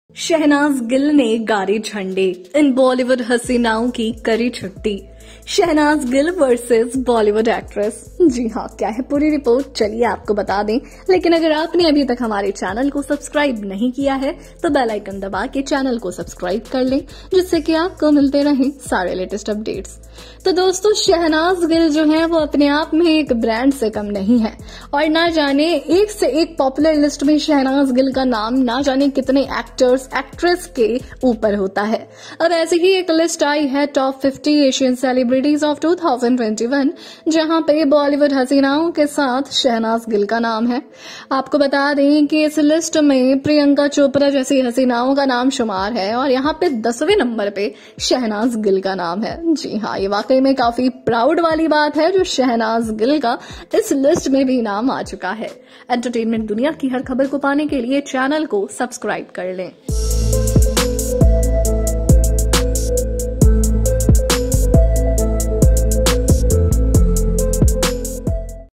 Shehnaaz Gill के आगे नहीं टिक पाई ये Bollywood हसीनाएं ! Top 50 Global Asian Celebrities
Bollywood's Desi Girl Priyanka Chopra Scared Of Shehnaaz Gill | Shehnaaz Gill & Priyanka Chopra In A List | Shehnaaz Gill Latest News And Updates

EDITOR :-- POP